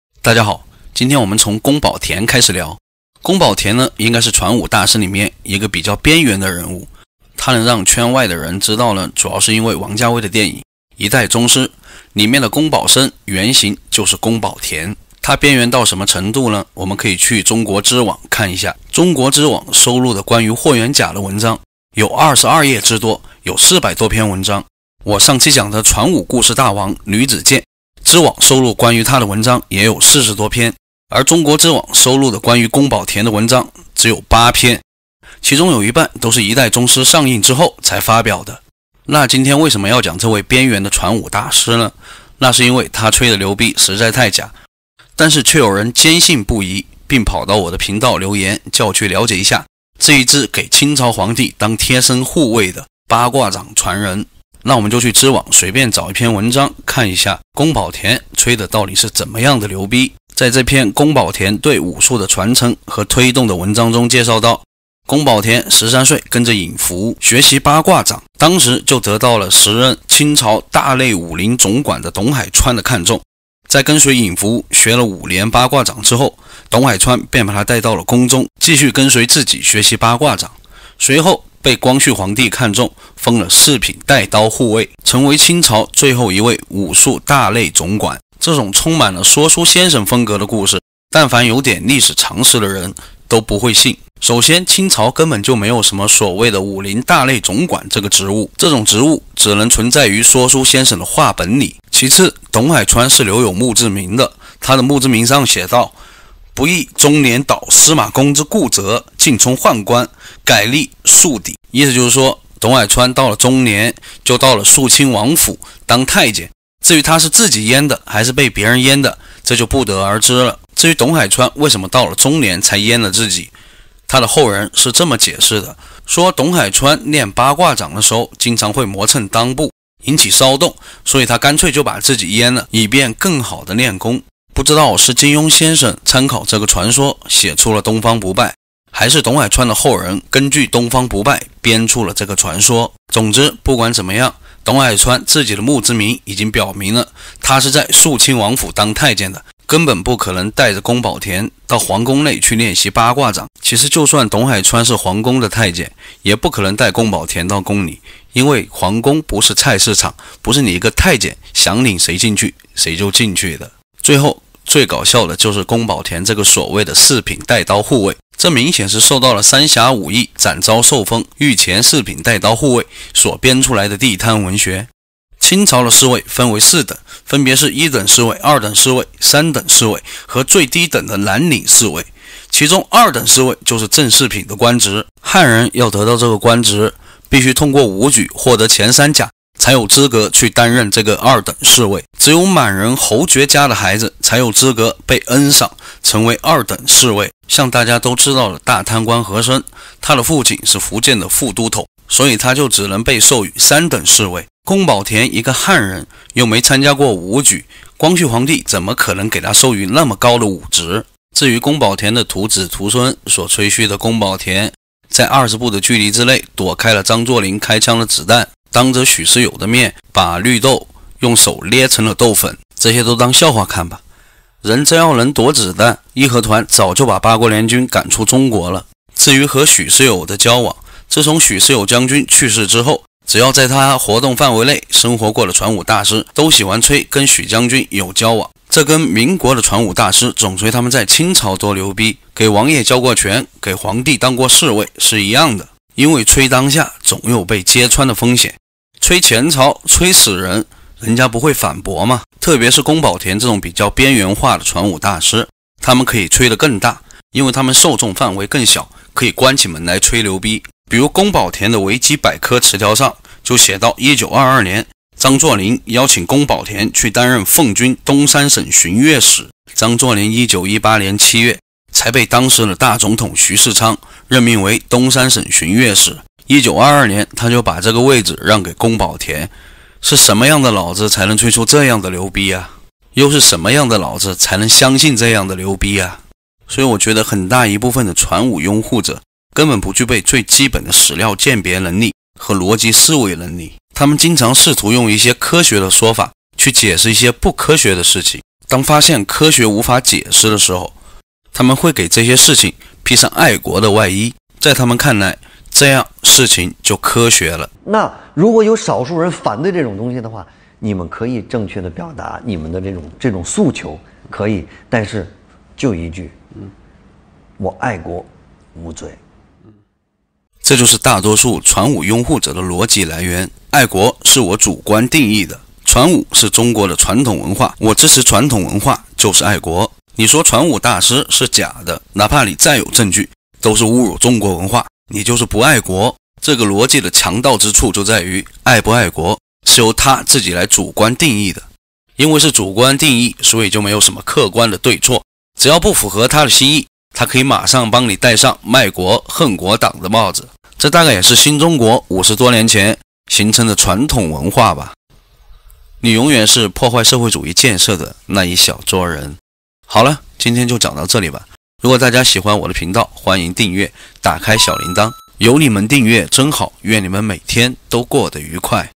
【瞎聊】自吹成為東三省巡閱使，能躲避子彈的傳武大師 宮寶田 | 中年自宮當太監的傳武大師 董海川
大太監董海川和他的傳人們
本頻道推薦